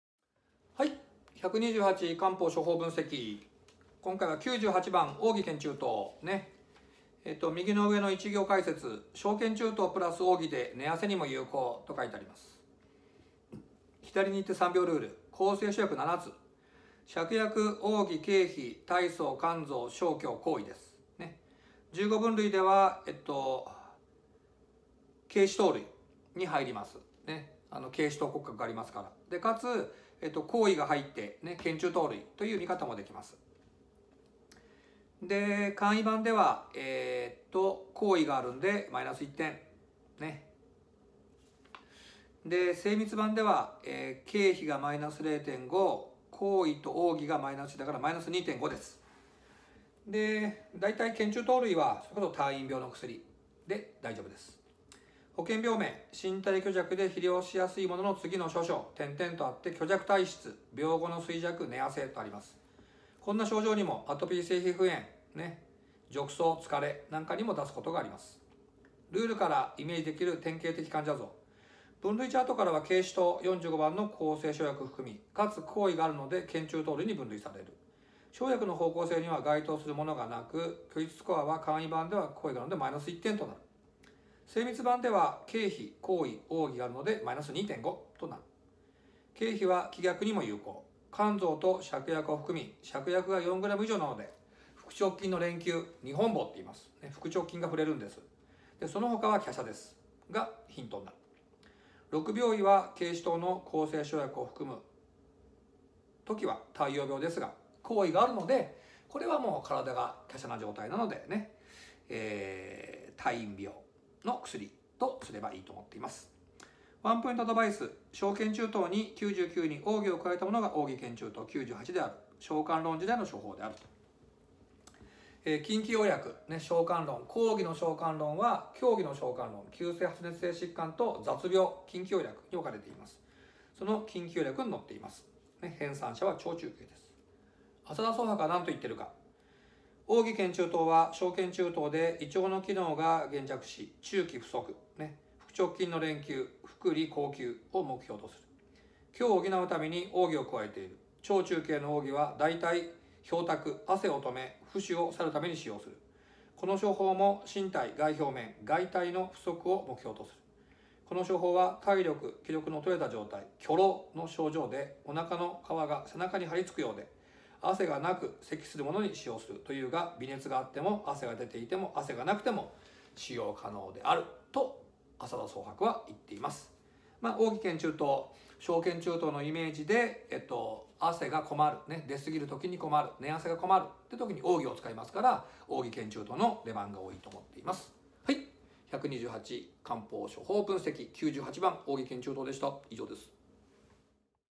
【漢方.JP】黄耆建中湯98〜実践3秒ルール 128漢方処方分析【新見正則が解説】
「実践3秒ルール 128漢方処方分析」黄耆建中湯98の解説です。医師の方が臨床の現場で使いやすいように、知識と実践をなるべくわかりやすくお話しします。

参考書籍１）
実践3秒ルール 128漢方処方分析 (3秒でわかる漢方ルール)

参考書籍２）
3秒でわかる漢方ルール

お話：新見正則
・漢方医＆外科医
・Doctor of Philosophy,Oxford Univ.
・2013年イグ・ノーベル賞受賞